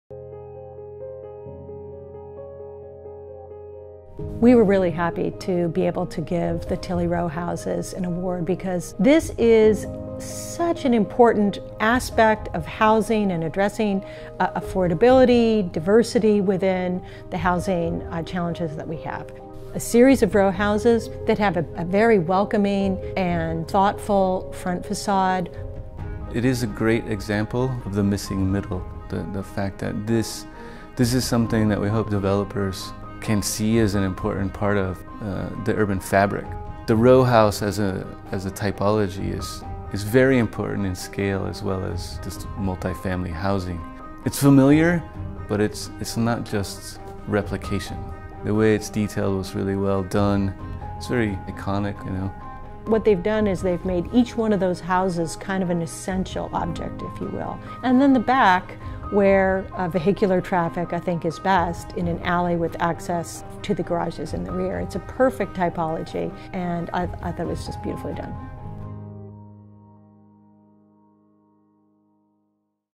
2017 AIA Austin Design Award - Tilley Row Houses by Michael Hsu Office of Architecture
Tilley Row Homes is an eight unit, five-star Austin Green Building rated townhome project located in the Mueller development east of downtown Austin. The development serves to help fill the ‘missing middle’ housing between single family and larger multi-family developments. At 1,800 square feet each, the houses are compact, but the repetition of forms created an economy of scale allowing quality materials and architectural details possible.

The typically traditional three gabled roofs are modernized in the removal of the overhangs, cladding in white stucco, addition steel and glass balconies and rich material details like warm wood shingles and colorful tile.

Each unit had finishes tailored to the homeowner. The inspiration for the interior design is a modern farmhouse with color feature pops of blue and blush on a crisp white backdrop. Each unit includes a tall bookcase with cable railing that doubles as a guardrail and screen for the stairway.